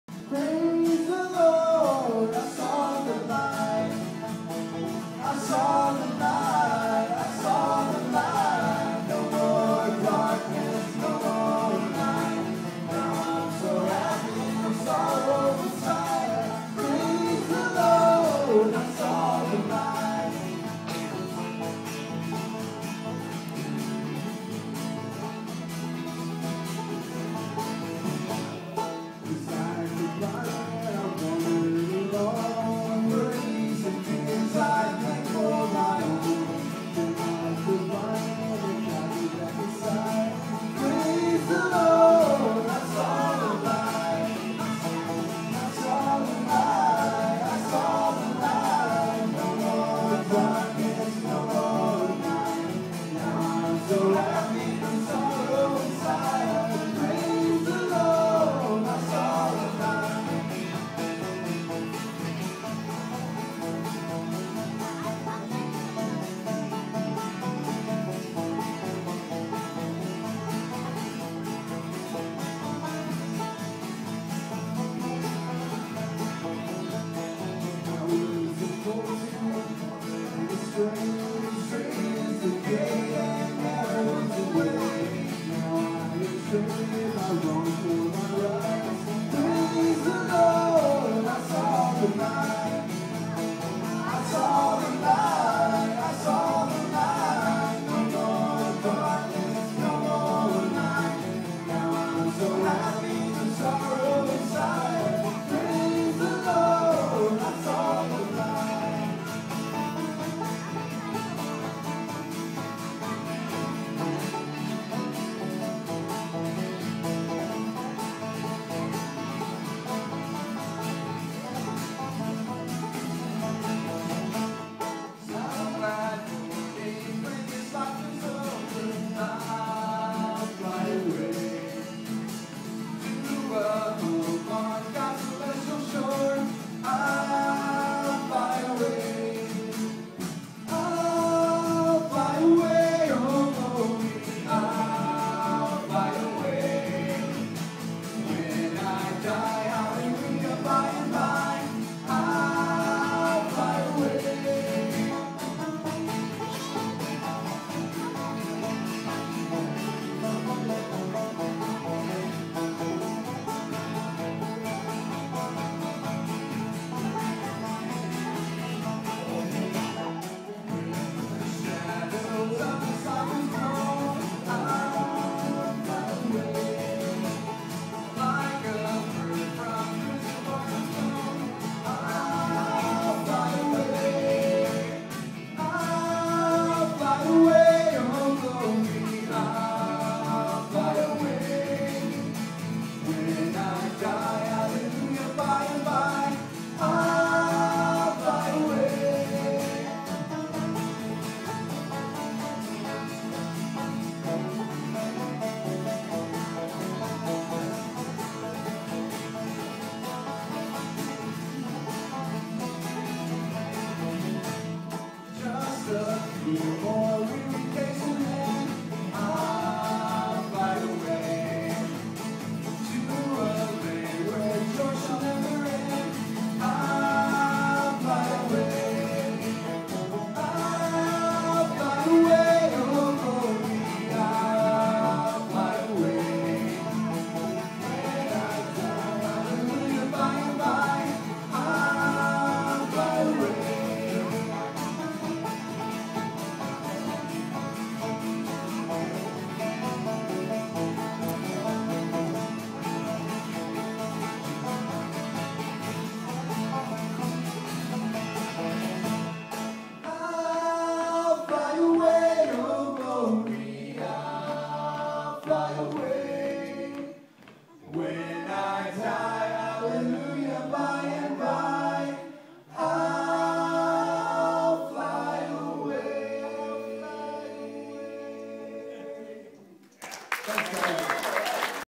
Dave & The Disposables
Playing a little bluegrass at "Tunes In The Trees"